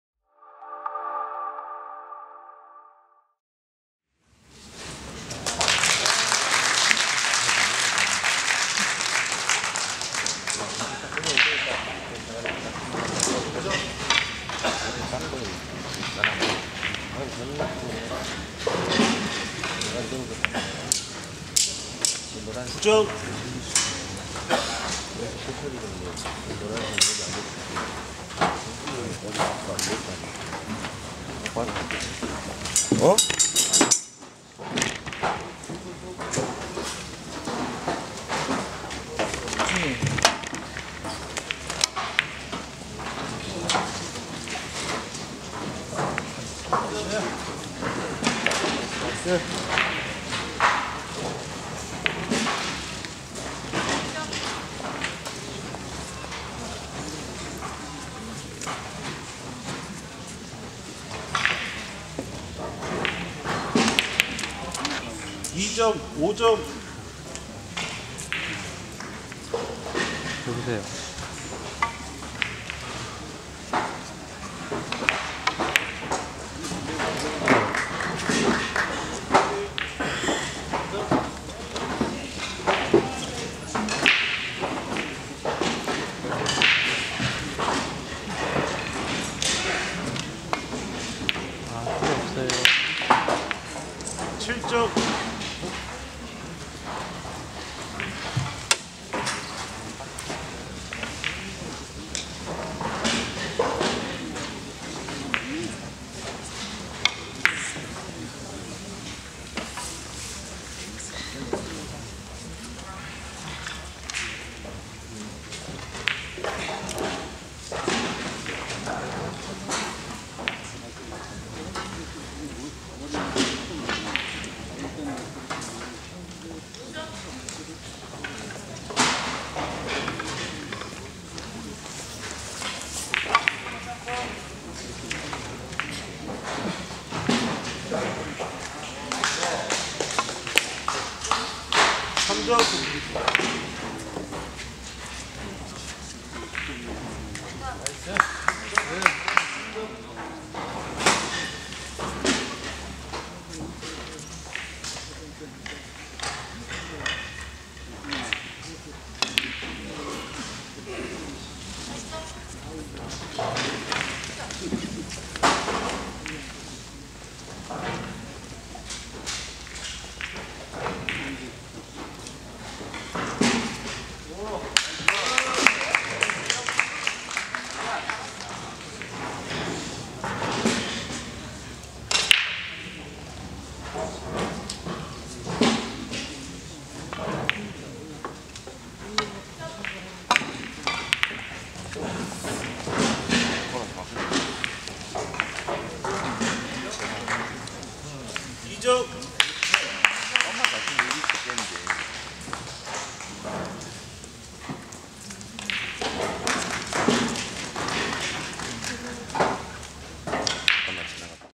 황철호 김규태 잉글리시 빌리아드 English billiards Korea Hwang chul-ho Kim kyu-tae
2013 Incheon national sports festival
 English billiards Korea Hwang chul-ho Kim kyu-tae

2013 인천 전국체전
잉글리시 빌리아드, 황철호 vs 김규태